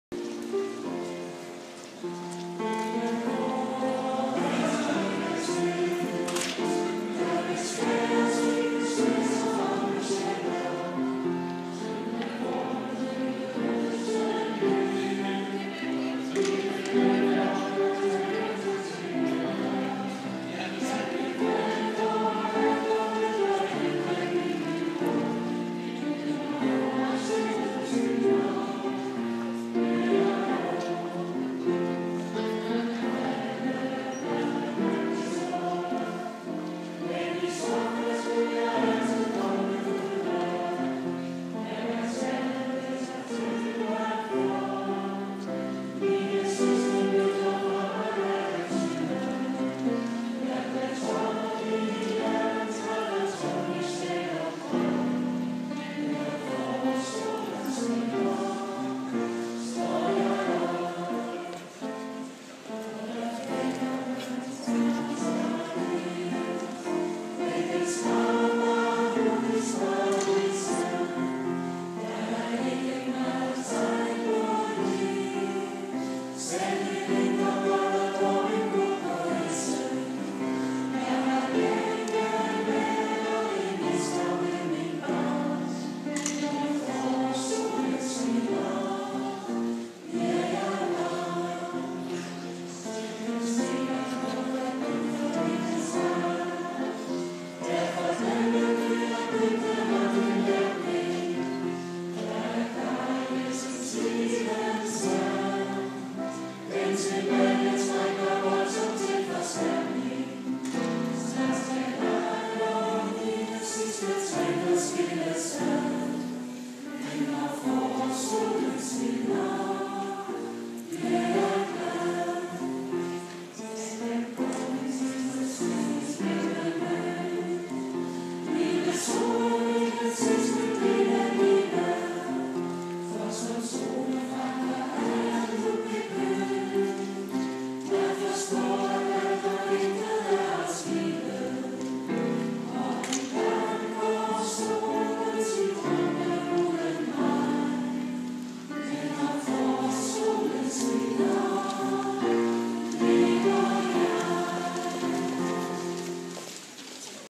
Morning Song at humanities faculty KUA2, 3
The third video of music students and springtime cheer with students and staff for a morning serenade. 7 March 2014, University of Copenhagen